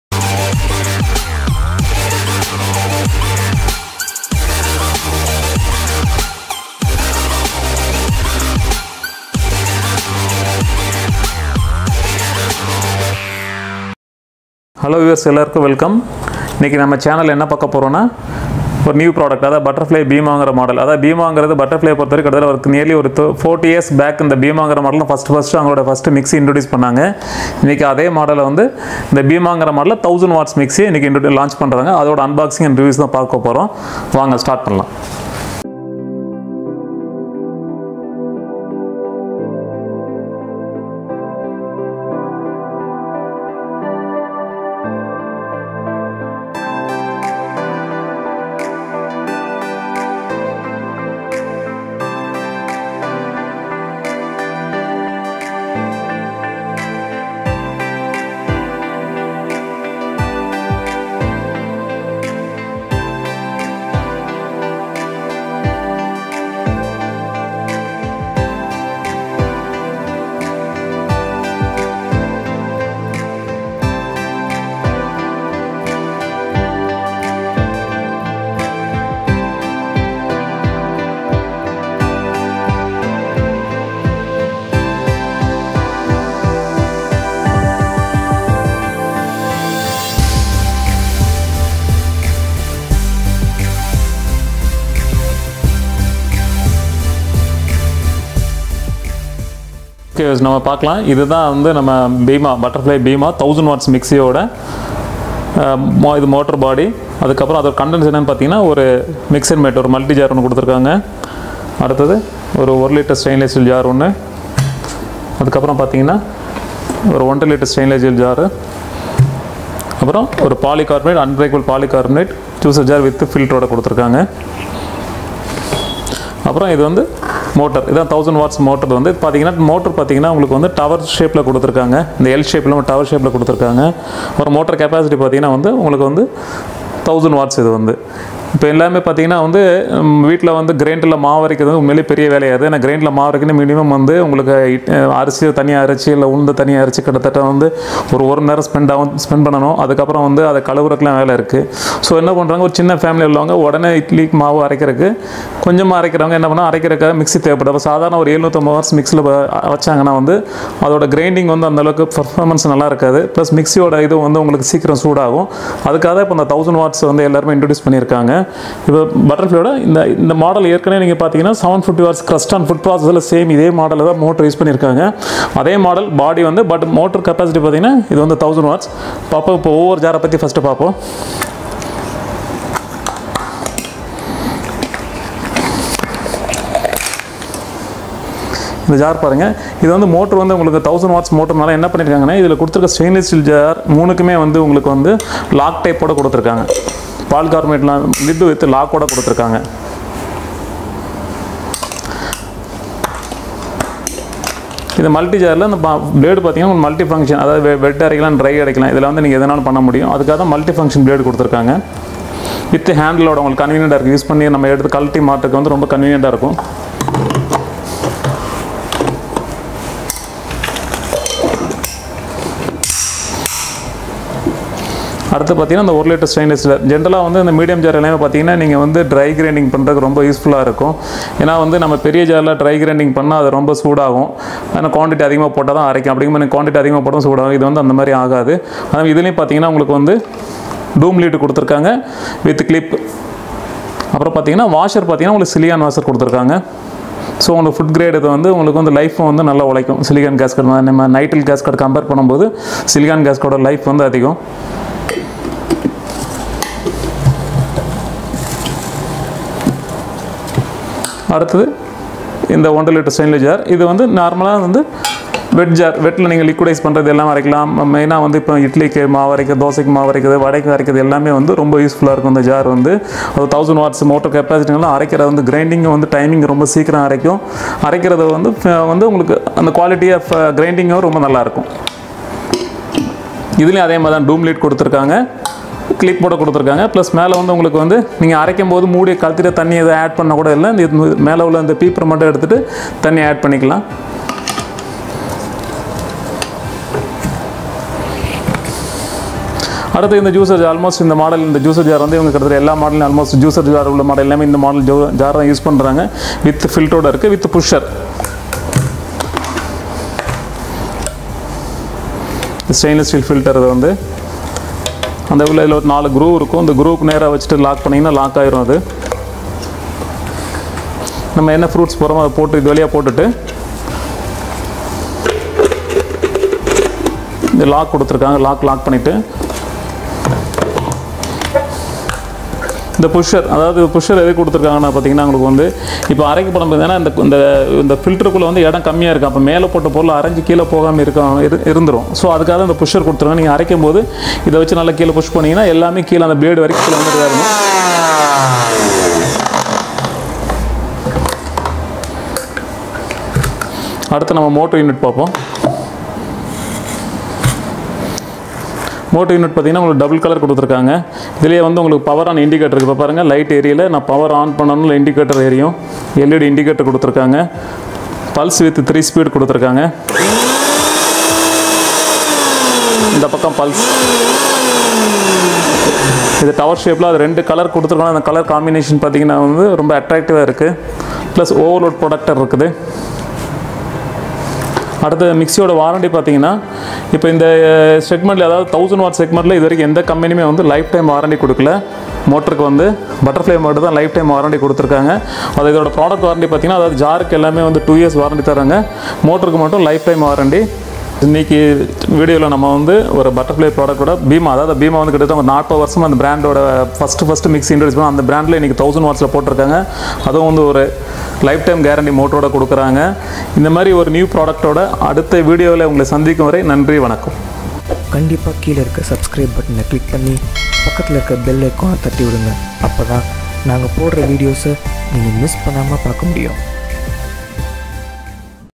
Butterfly Bhima 1000W Mixer Grinder Lifetime warranty ஆஆஆ!!| Unboxing & Review | Tamil
1000W Heavy Duty Motor for Tough Grinding Operations, Non-slip Feet - Yes
Premium Stainless Steel Jars for Wet & Dry Grinding Operations
LID Lick System in Jars for Hands free Operations
Transparent Lid for viewing & experiencing the Grinding Operations
Specially Designed Vacuum Rubber Boots for stronger Grip & Shake Free Operations
360 Degree Ventilation to Protect the Motor from Heating
ABS Body for better Durability
Unique Strong Blades for Wet & Dry Operations
Juicer Jar for Dual Operations – as Juicer & Blender
Over Load Protector for Motor Protection